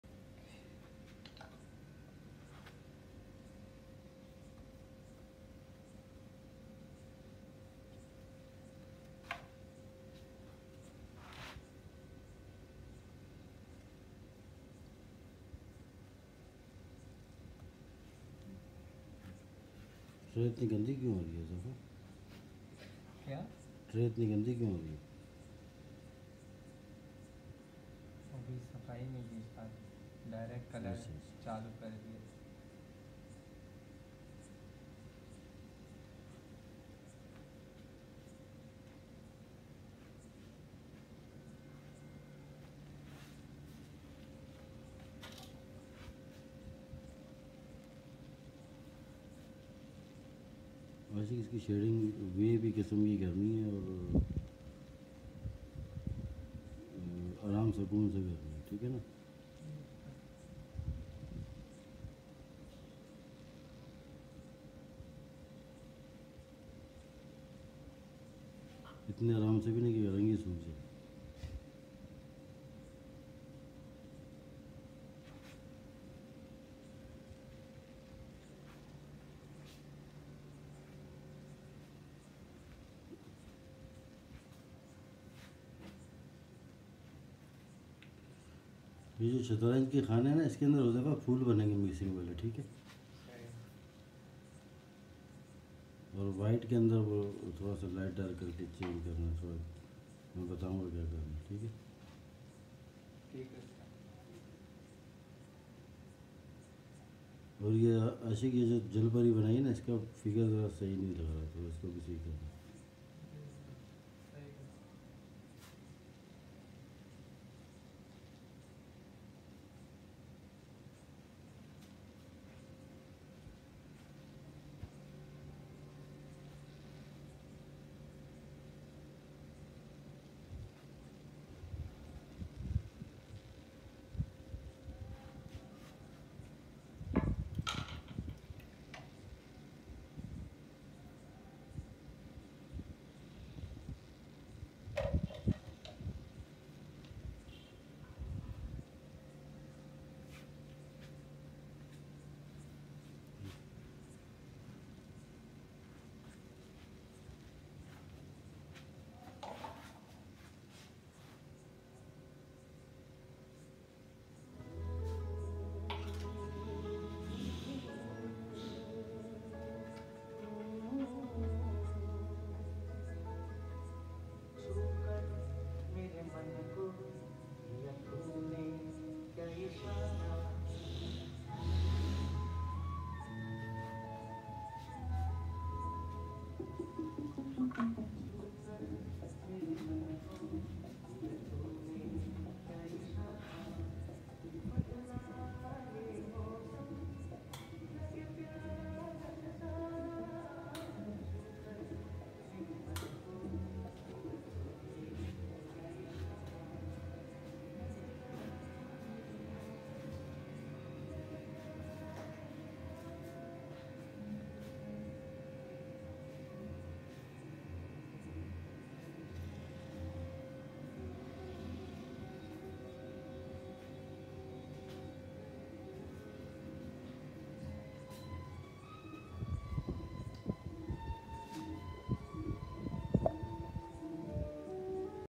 Truck art Academy international
Student’s attending classes in nightshift at Truck art Academy